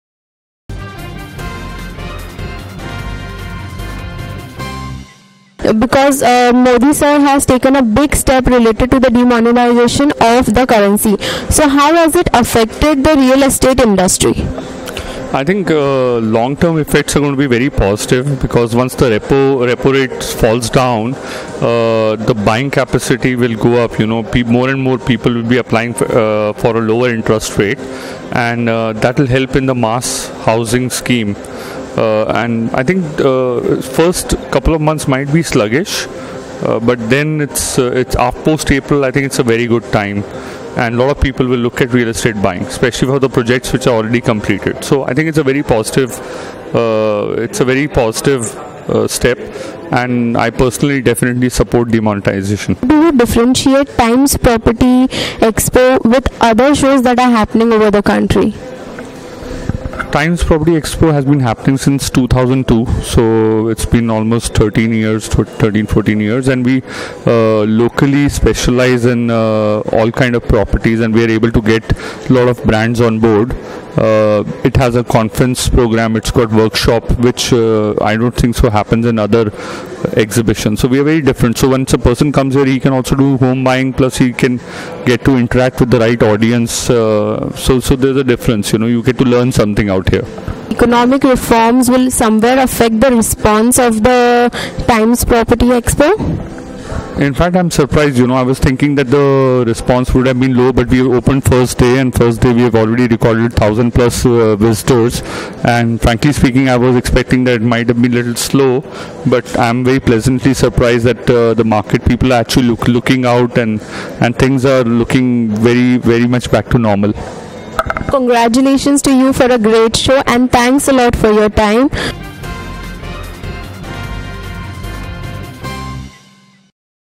Mr. Manish Gandhi interview with 9amstories.com
Mr. Manish Gandhi, COO & Executive Director, ABEC, talks about Times Property Expo, impact of Demonetization on Real Estate Sector and more with 9 AM Stories.